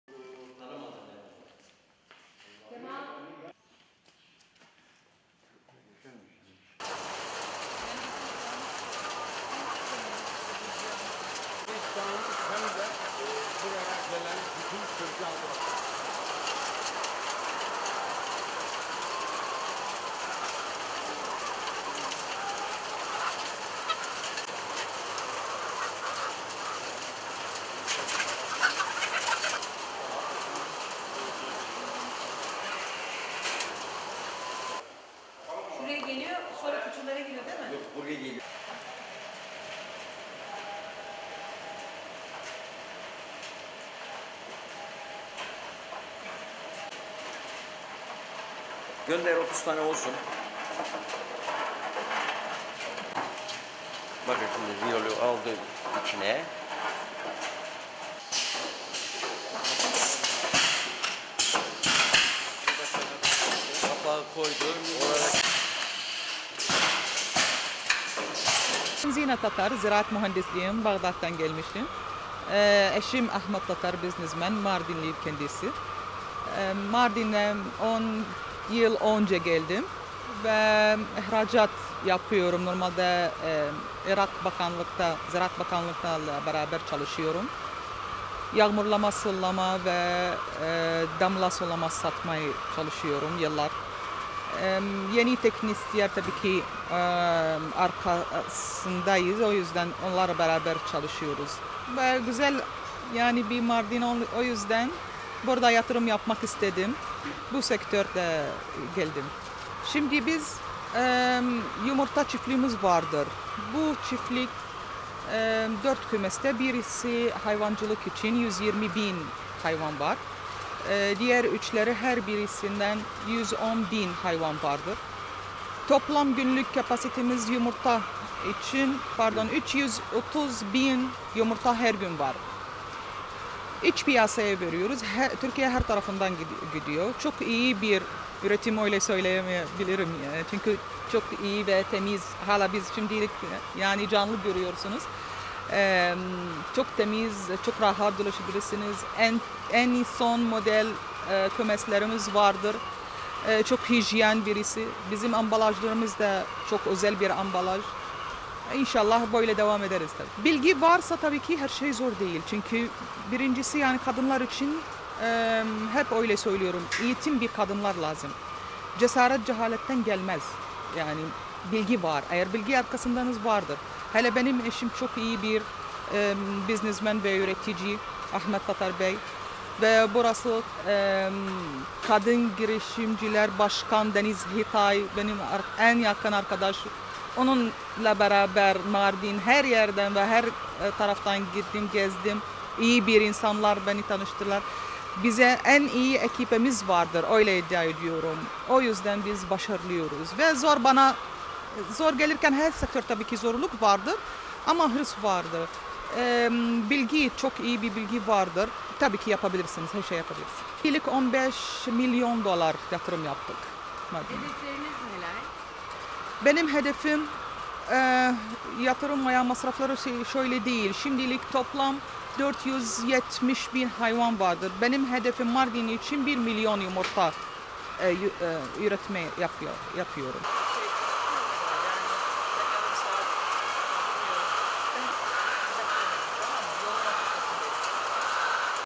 Irak’tan Geldi, Mardin’e 15 Milyon Dolarlık Yatırım Yaptı
Yaklaşık 10 yıl önce Irak’tan Mardin’e gelen Zine Tatar, devletten de aldığı destekle 15 milyon dolara tavuk çiftliği kurdu. Yem ve gübre fabrikası ile birlikte çalışmalarına devam eden Tatar, günde 330 bin adet olan yumurta sayısını 1 milyona çıkarmayı hedefliyor.

Kadın girişimci olarak Mardin’e yaptığı yatırımla diğer kadınların takdirini kazanan Zine Tatar’ın Mardin macerası 10 yıl önce başladı. Irak’tan Mardin’e gelen Tatar, Yeşilli ilçesinde 15 milyon dolar harcayıp devlet desteği de alarak tavuk çiftliği kurdu. Günde 330 bin yumurta elde eden Tatar’ın en büyük hedefi bu sayıyı günde 1 milyona çıkarmak.

Bağdat’ta ziraat mühendisliği eğitimi aldığını ve Irak Tarım Bakanlığında üst düzey yöneticilik yaptığını belirten Zine Tatar, yağmurlama, sulama, damla sulama yöntemleri ile ilgilendiğini söyledi. Mardin’in güzel bir şehir olduğunu ve burada yatırım yapmak istediğini kaydeden Tatar, “Yumurta çiftliğimiz vardı. Toplam günlük yumurta kapasitemiz her gün 330 bin yumurta üretimi yapıyoruz. Çok iyi bir üretim, iç piyasaya veriyoruz. Çok hijyenik bir şekilde devam ediyoruz. Kadınlar için eğitim lazım, cehalet cesaretten gelmez. Eşim çok iyi bir yönetici, Kadın Girişimciler Başkanı Deniz Giray Mardin’de bana çok destek oldu. Her işte zorluk vardır, hırs vardı ama bilgi vardı. 15 milyon dolarlık yatırım yaptık. Benim hedefim 470 bin hayvanımız var, hedefim Mardin’de günlük 1 milyon yumurta üretimi yapmak” dedi.

“Hibe ve projeler doğru yerde kullanılmalı”

Kurdukları entegre tesiste her şeyin otomatik olarak işlediğini anlatan Tatar, şöyle devam etti: “Tavuk kümesleri için sağlıklı yem kullanma zorunluluğu var. Bunun için yem fabrikasını kurduk. Saatte 15 ton üretim yapabilen bir fabrika. Biz günlük 45 ton yem kullanıyoruz. Yemler el değmeden makine sistemi ile tavuklara belirlenen şekilde aktarılıyor. Bunun yanında tavukların gübrelerini de değerlendiriyoruz. Bunun için de fabrika kurduk. Tavukların bulunduğu kümeslikten dışkıları otomatik olarak gübre fabrikasına gönderiliyor, kurutulduktan sonra işlenerek paketleniyor. Çok değerli organik bir gübre elde ediyoruz. Devletin hibe ve destek projeleri doğru yerde kullanılırsa ülke ekonomisine büyük katkılar sağlanmış olur ve istihdam alanları genişler.”